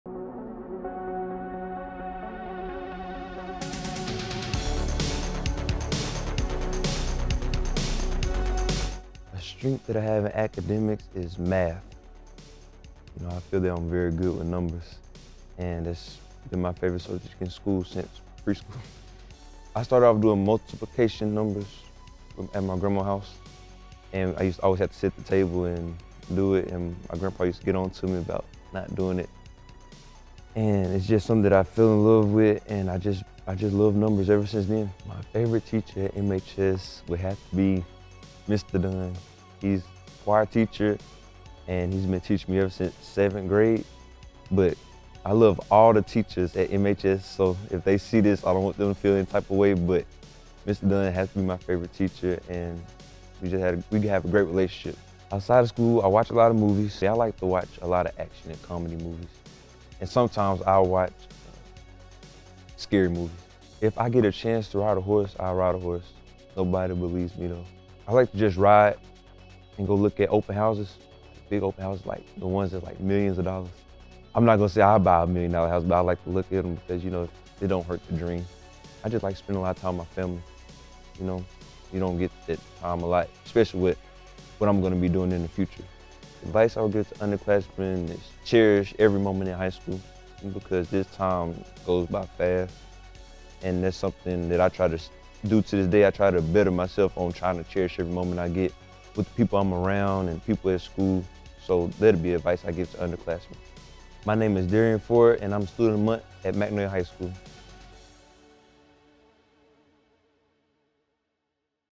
MHS Student of The Month
Panthers on Point Presents Derrian Ford - Student of the Month